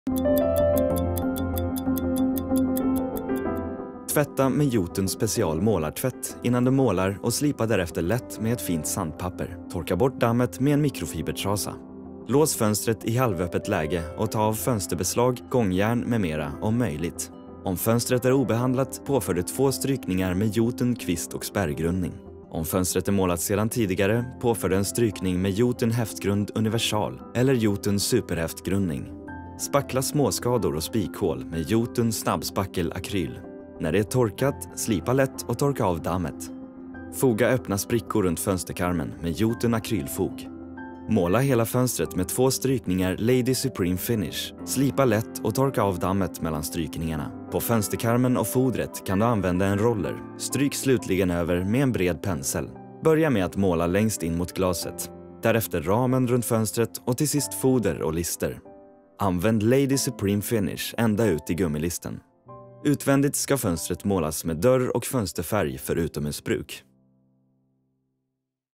Måla fönster inne
Så här gör du för att måla fönster och fönsterkarmar inne. Konkreta tips på 60 sekunder.